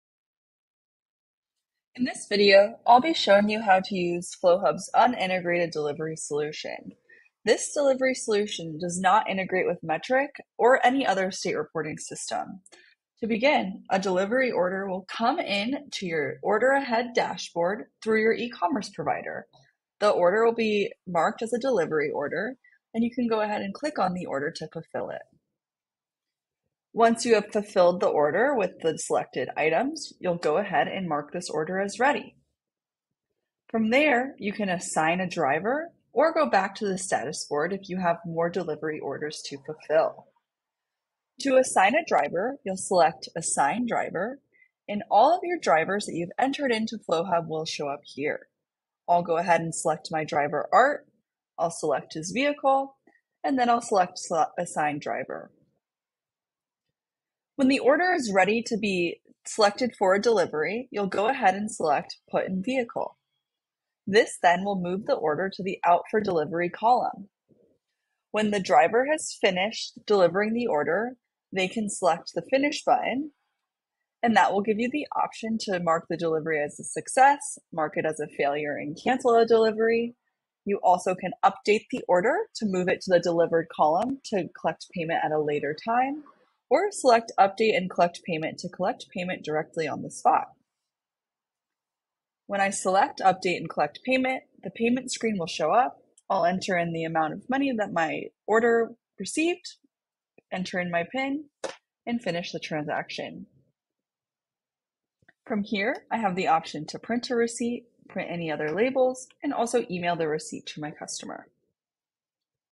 How To Use Flowhub Exclusive Delivery
Learn how to use Flowhub's in-app delivery workflow in this short video tutorial.

Flowhub is the dispensary management and payments platform that helps retailers stay compliant, streamline operations, and sell more cannabis with full flexibility and control.